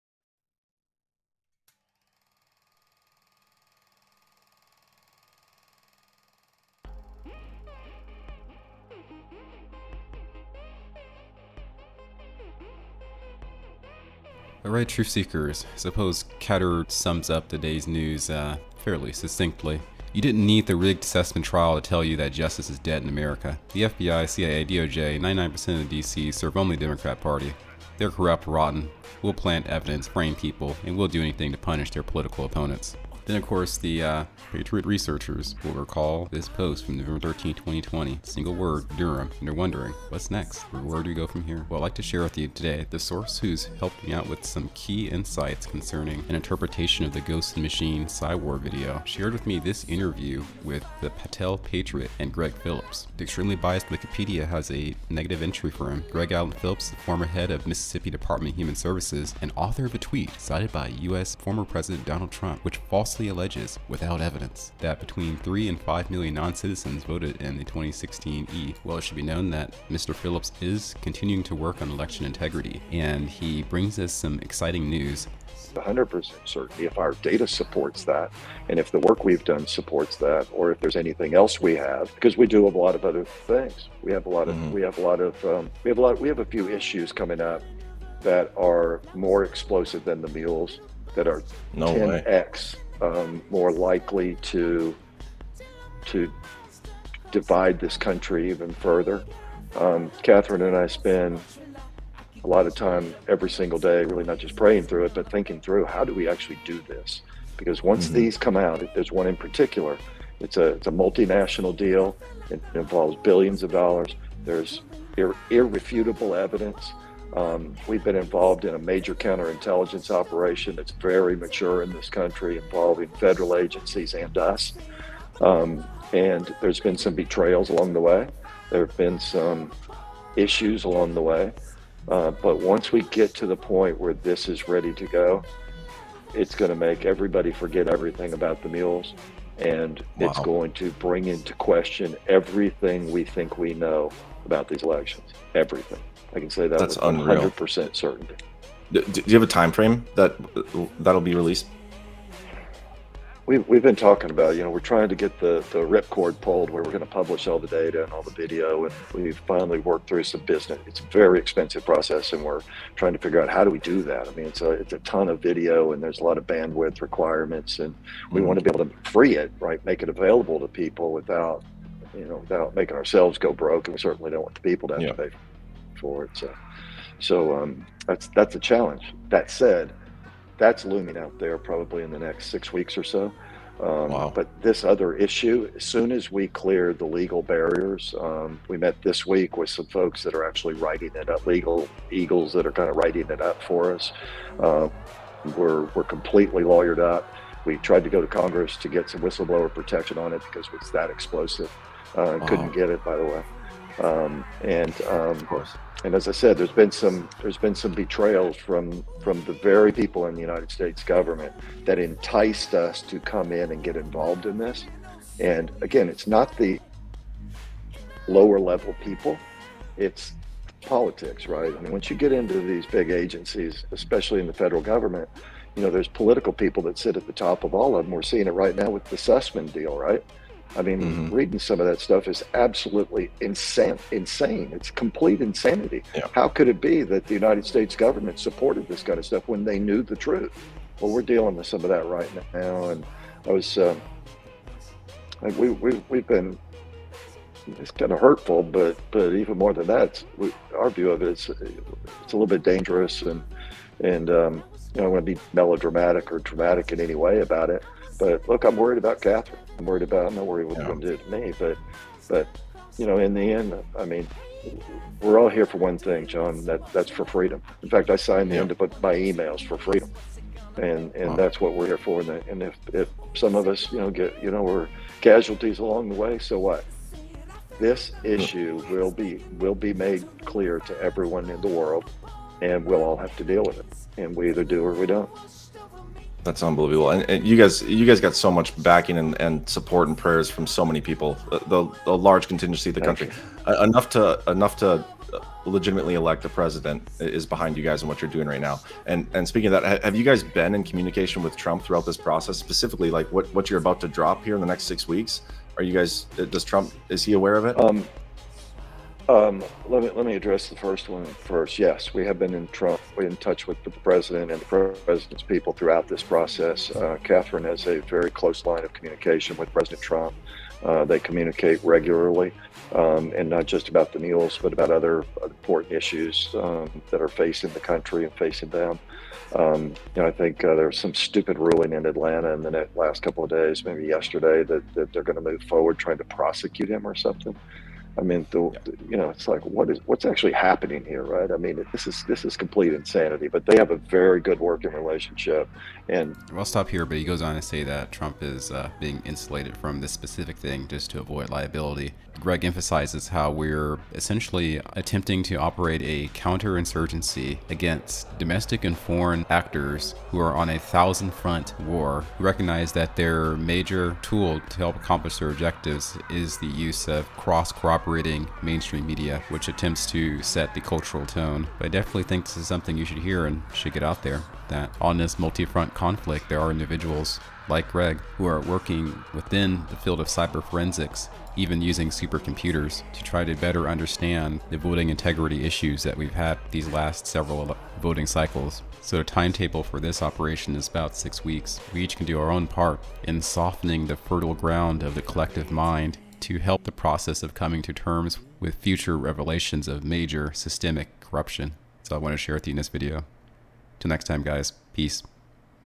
6 WEEKS until Major US ELECTIONS Forensics Announcement
Gregg Phillips indicates there are more bombshells to come with video evidence that will forever change how the US public looks at the election cycles.